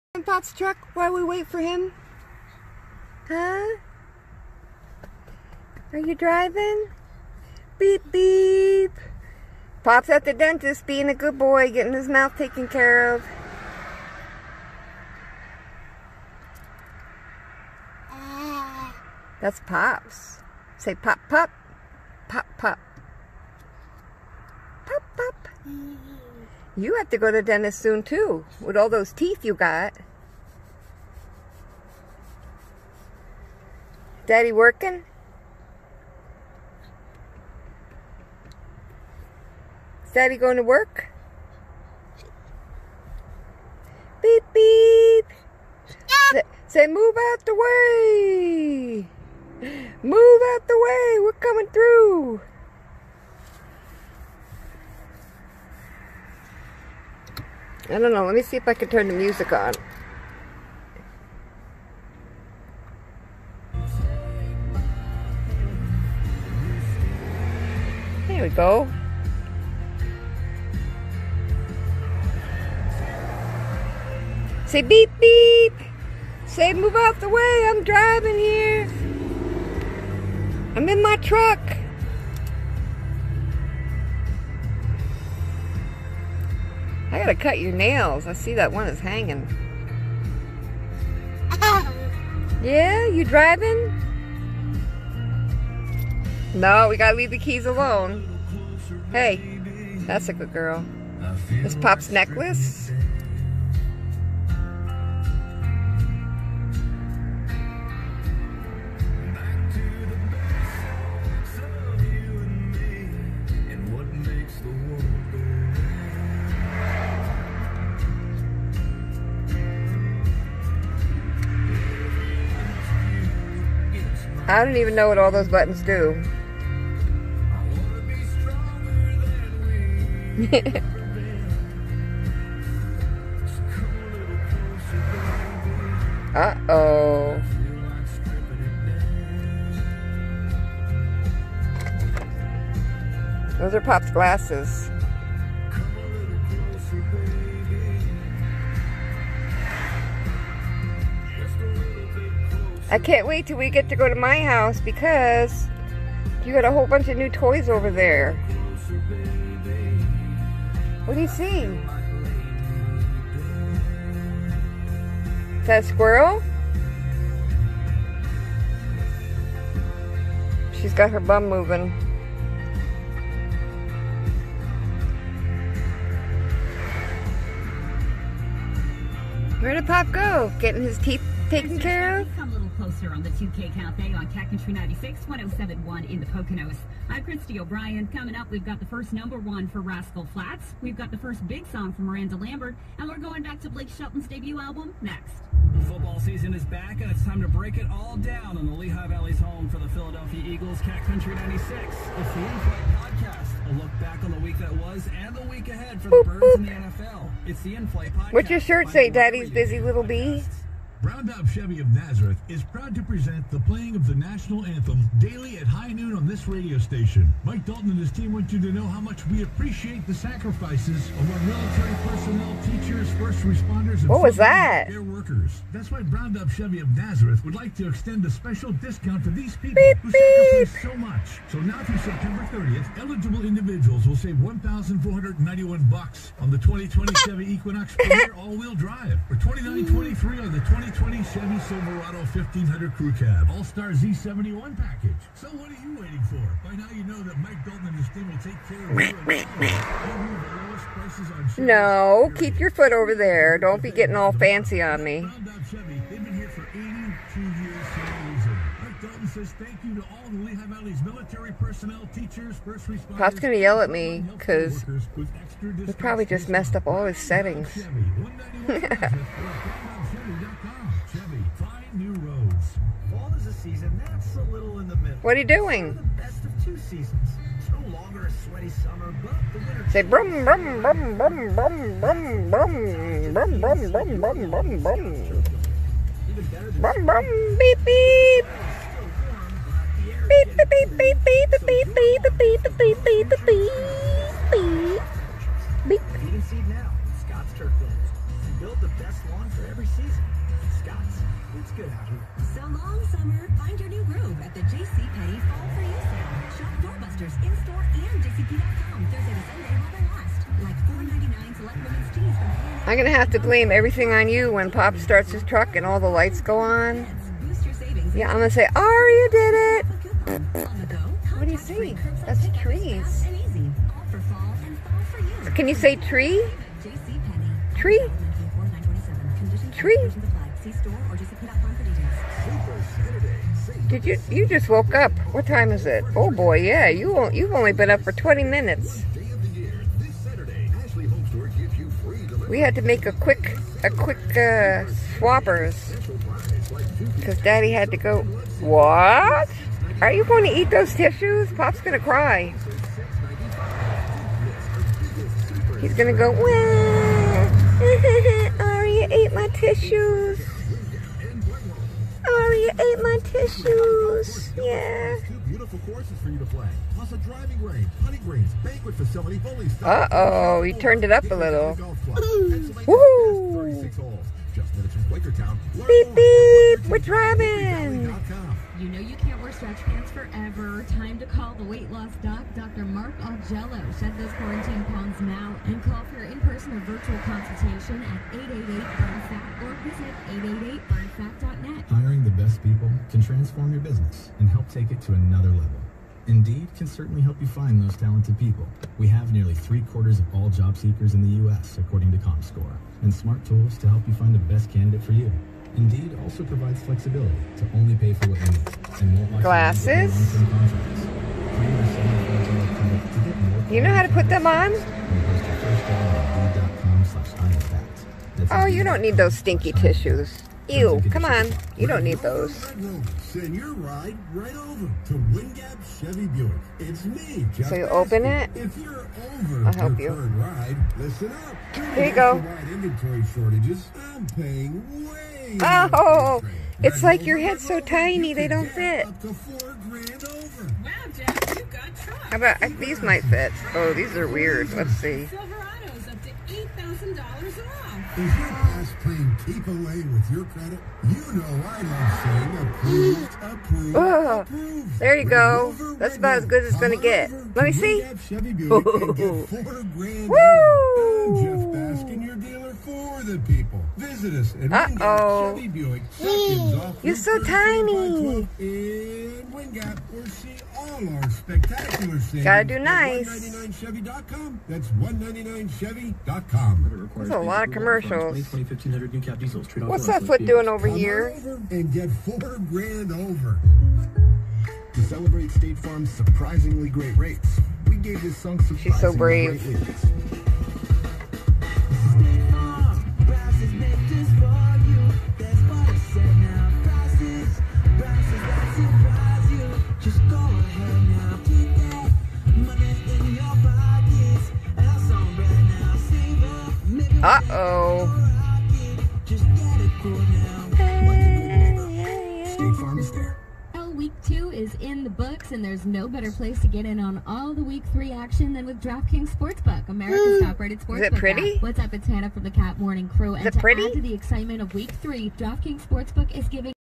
Aria taking driving lessons...
Just waiting for Pop while he's getting his teeth worked on.
   Dad's working ♡